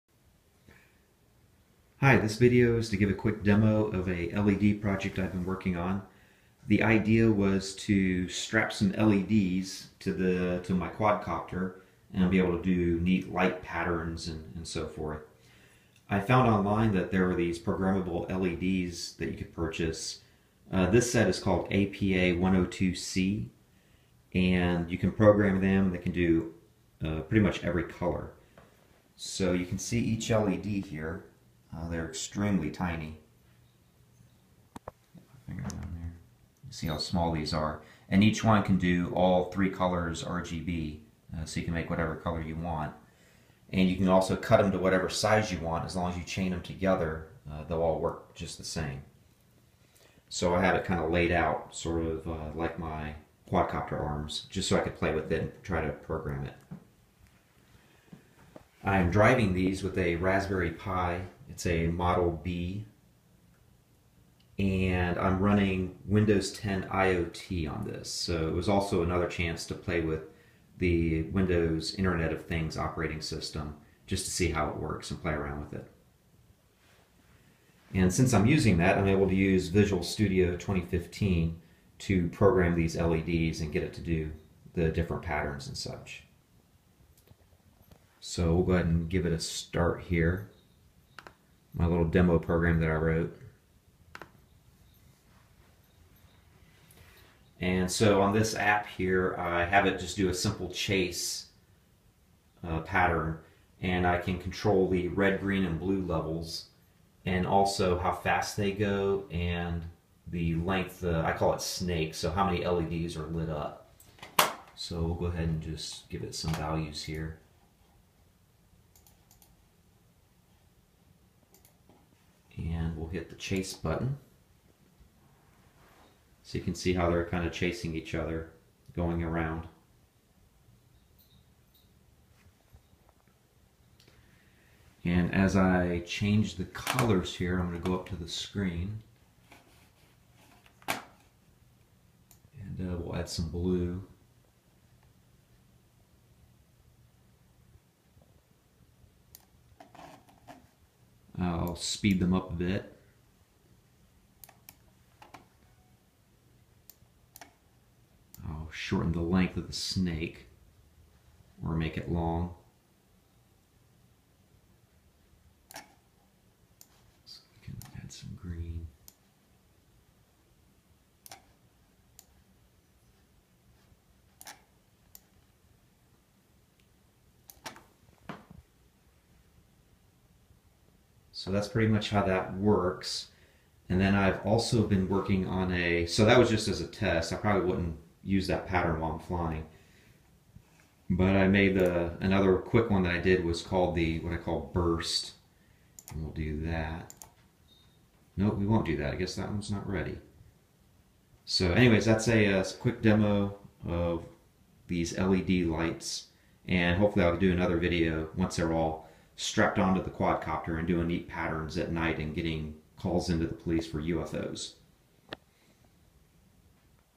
LED Project Windows 10 IoT
LED strip project using apa102C LED's and Windows 10 IoT for Raspberry Pi.